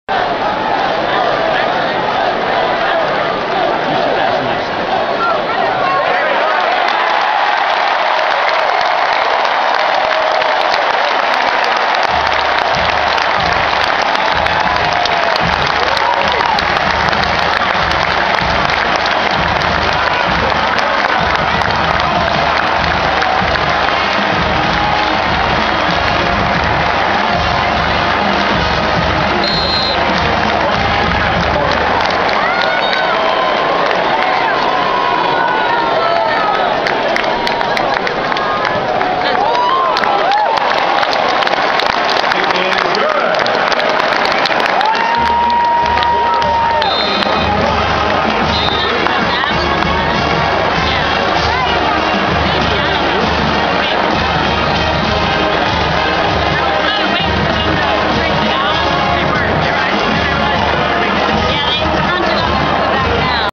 Carlos Hyde Touchdown vs. Miami(OH) 9/1/12
2nd quarter Carlos Hyde 4 yard TD run